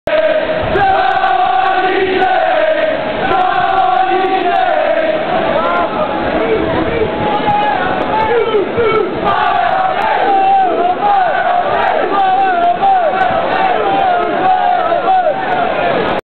COYS BAWA
Supras flag display at first match for St Mirren in new stadium against Kilmarnock 31/01/09.